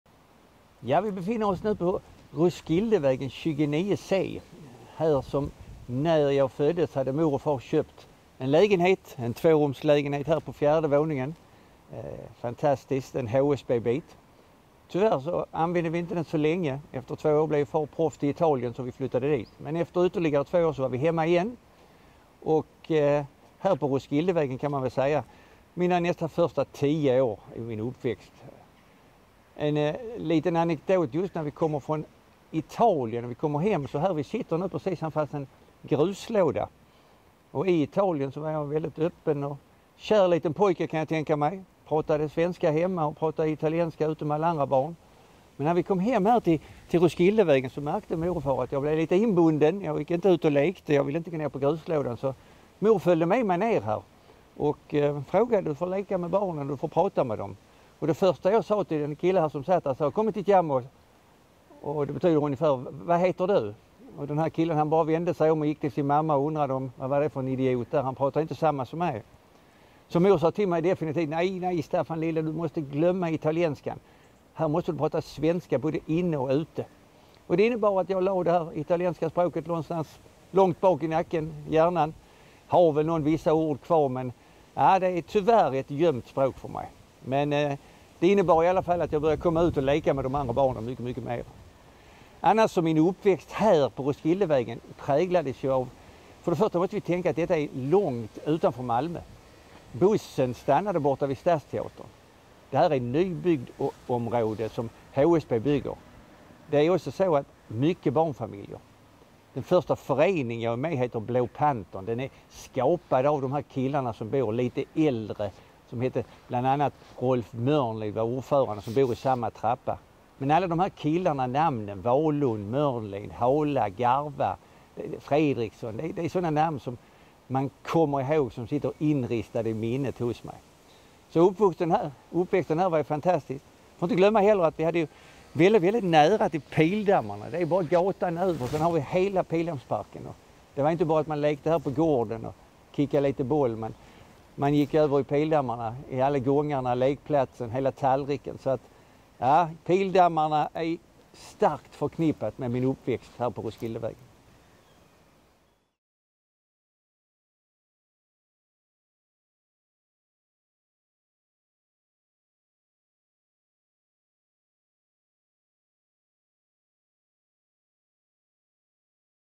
Staffan Tapper. En rundvandring i mitt liv. Del 7: Roskildevägen
I en rad filmer får vi träffa den legendariske fotbollsspelaren Staffan Tapper när han vandrar runt i olika delar av Malmö som haft betydelse för hans liv. Boken "En rundvandring i mitt liv" släpptes den 15 juni 2023 i Vinterträdgården, Malmö stadion.
Film: Anders Roos